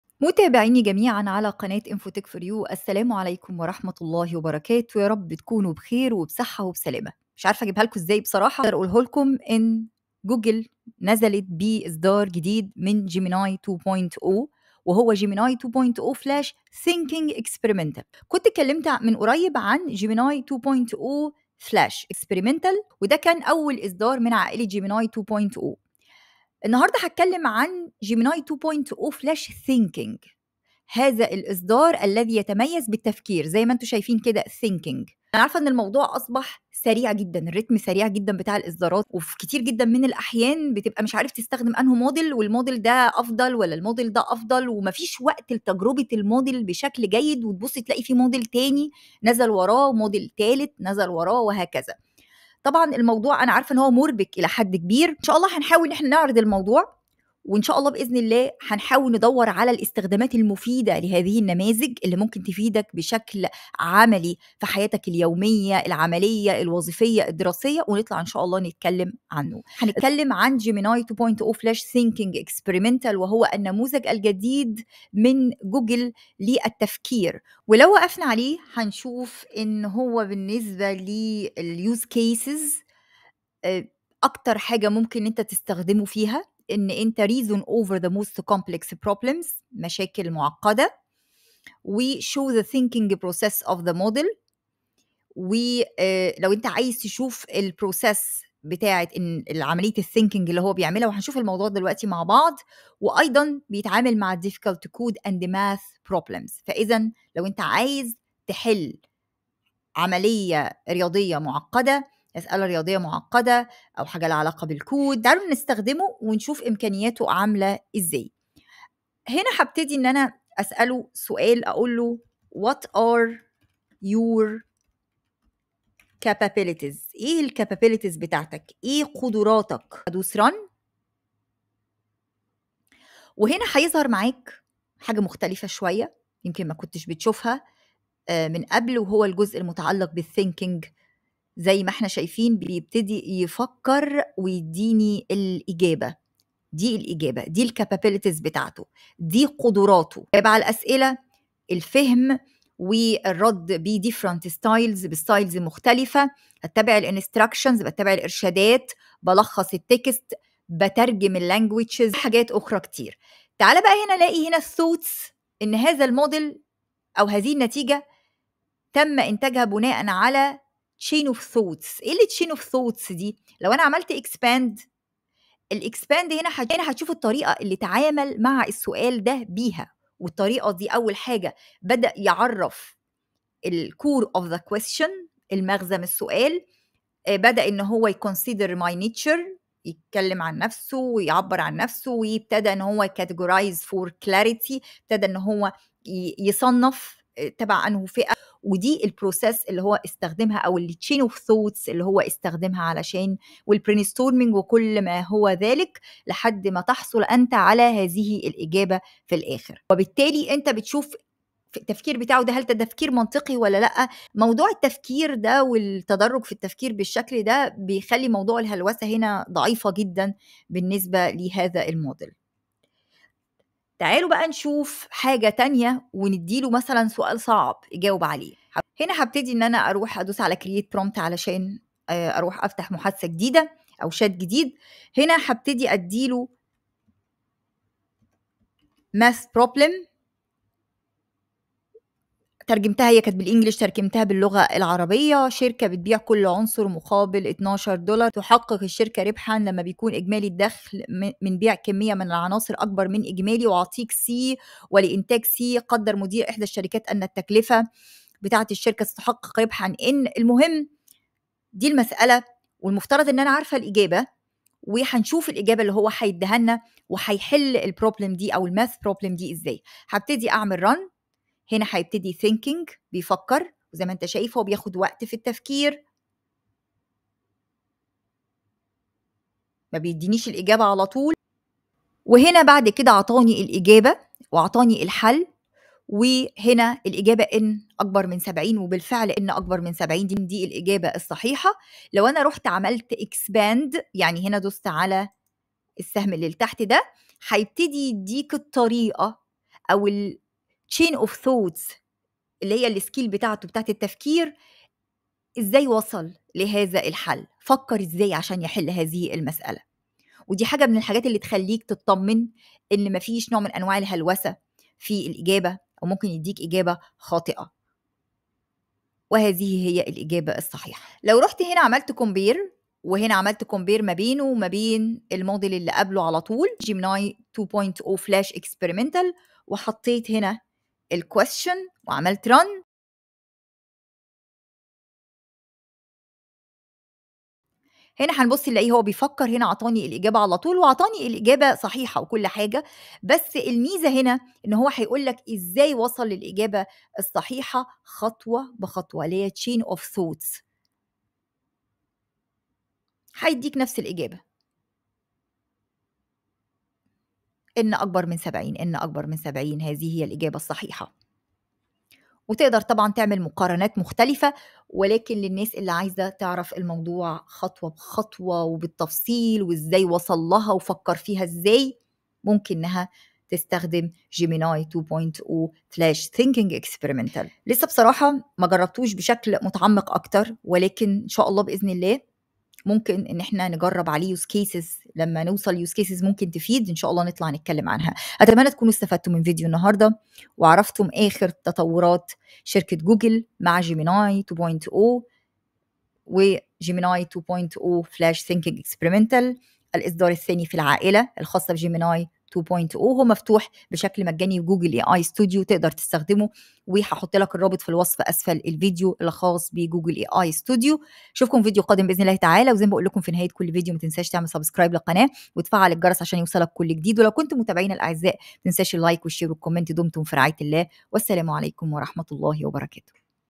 Gemini 2.0 Flash Thinking Exp نموذج يفكر من جوجل - تجربة عملية
في هذا الفيديو نستكشف نموذج Gemini 2.0 Flash Thinking التجريبي الذي تم إصداره حديثًا، والذي يوفر قدرات تفكير متقدمة. سنختبر النموذج الجديد، ومقارنته بنموذج الذكاء الاصطناعي السابق Gemini 2.0 flash.

✅ perplexity Spaces
perplexity Spaces